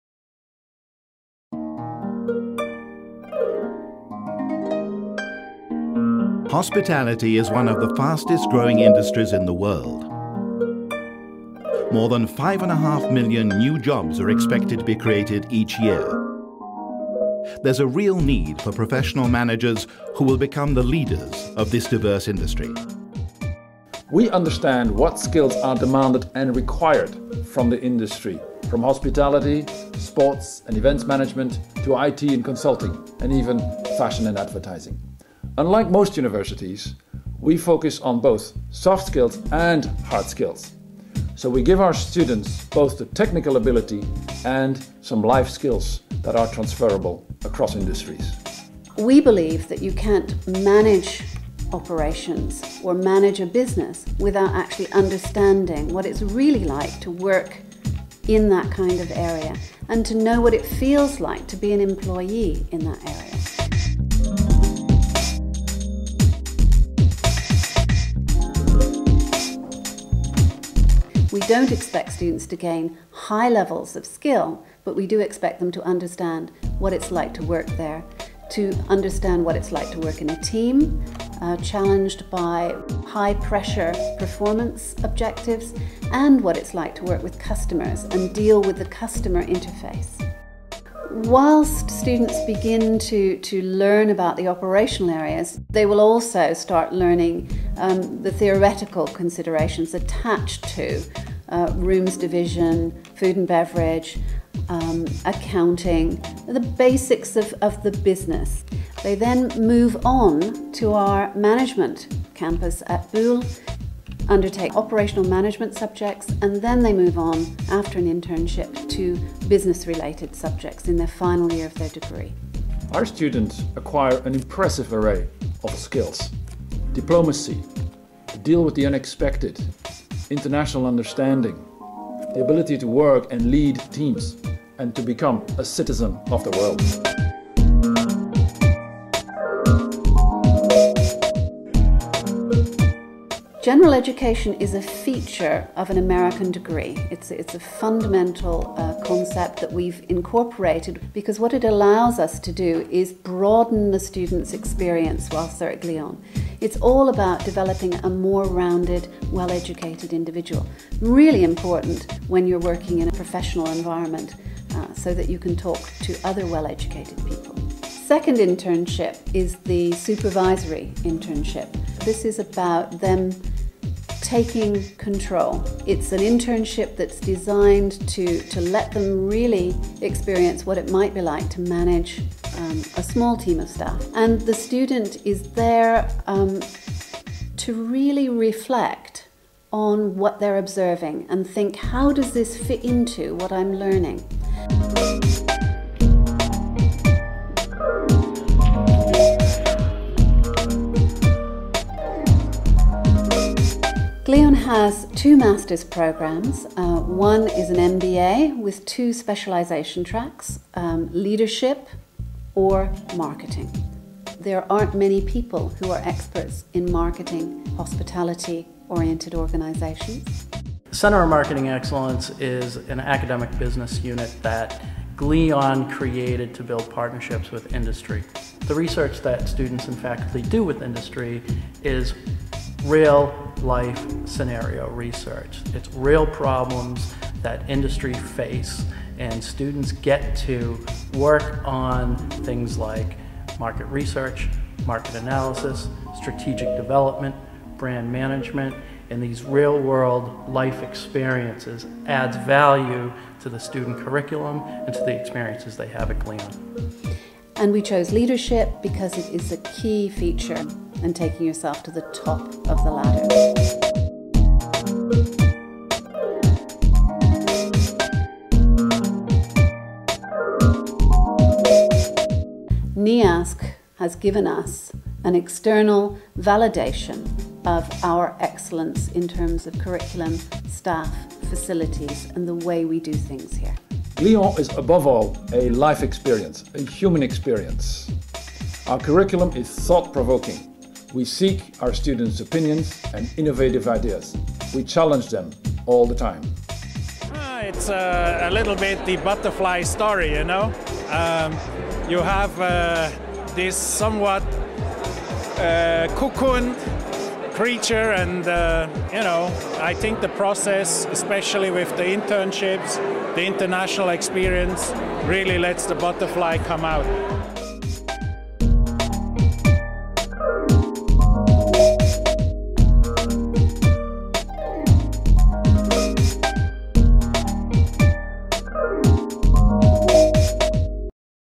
Glion Institute Montreux Riviera
Glion Institute of Higher Education, in Montreux Riviera is among the top 3 hospitality management schools in the world for studies in the fields of hospitality, hotel and tourism. Students come from all over the world to Montreux Riviera to study at this prestigious institution. cuckooco.com